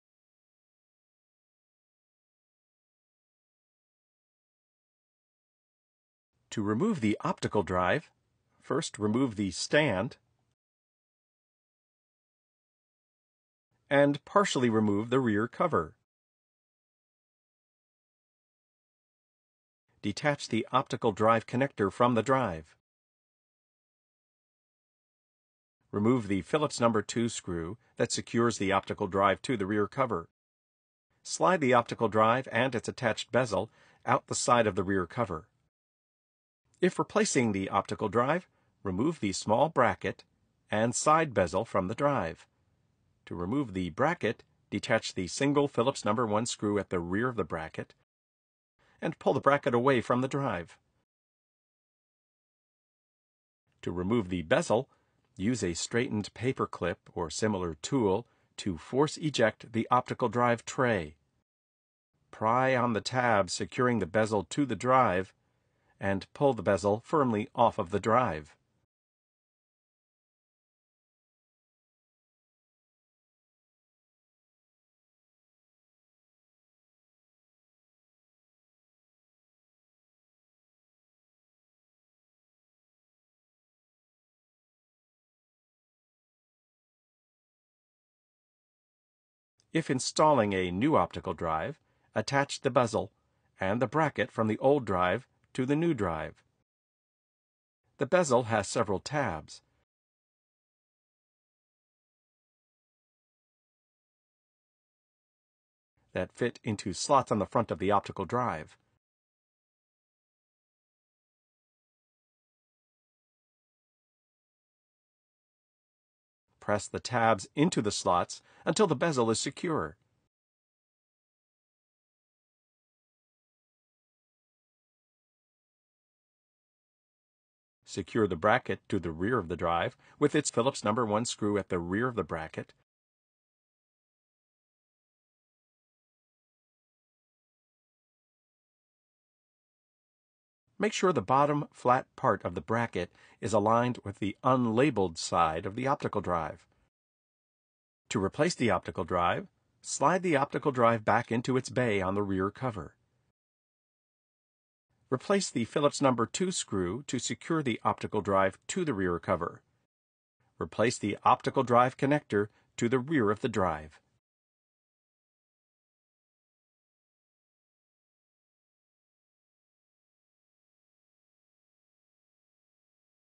Replace the Optical Drive | HP 22-b400 All-in-One Desktop PC Series
Learn how to replace the optical drive for HP 22-b400 All-in-One Desktop PC series.